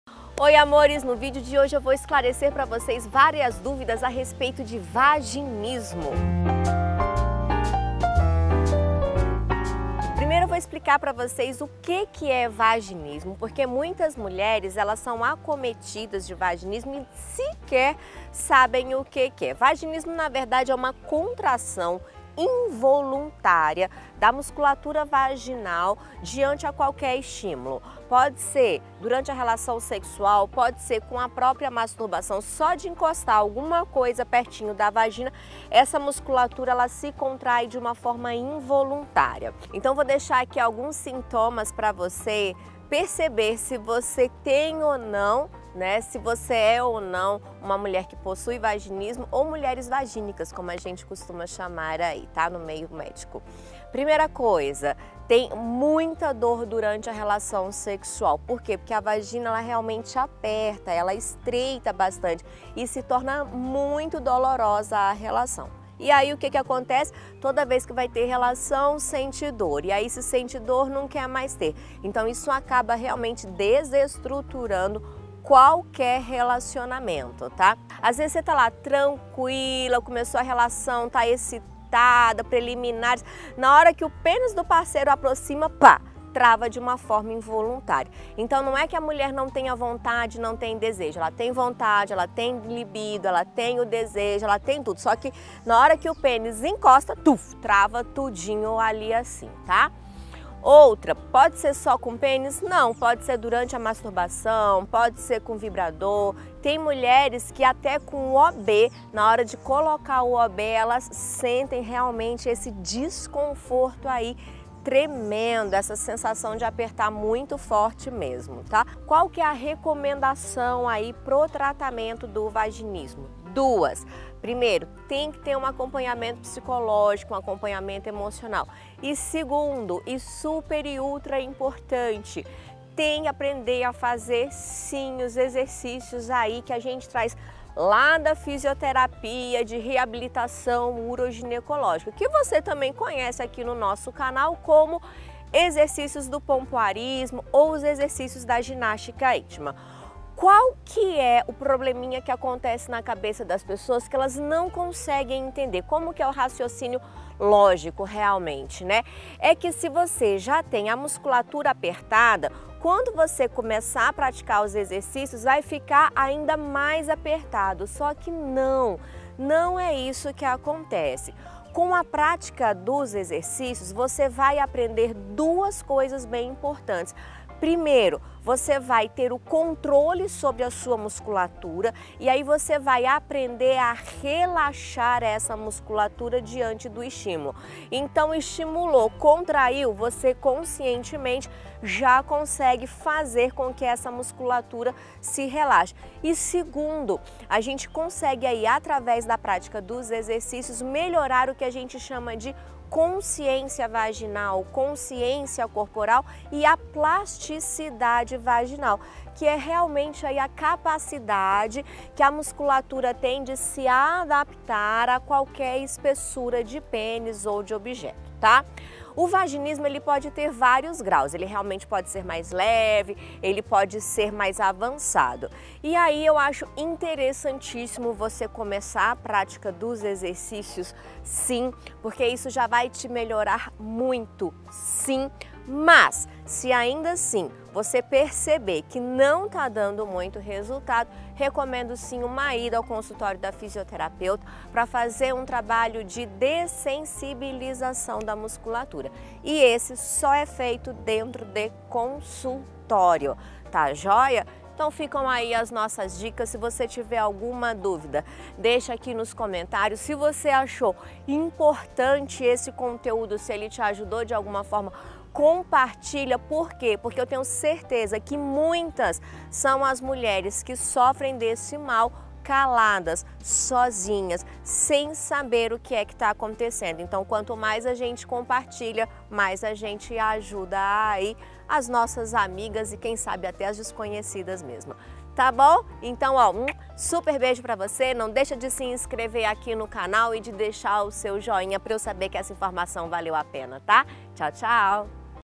Vaginismo: O que é? Porque tantas mulheres sofrem dessa disfunção?!
Aulão gratuito do meu curso de ginástica íntima dia 18/02 às 20h. Participe Descubra o poder das mulheres que praticam ginástica intima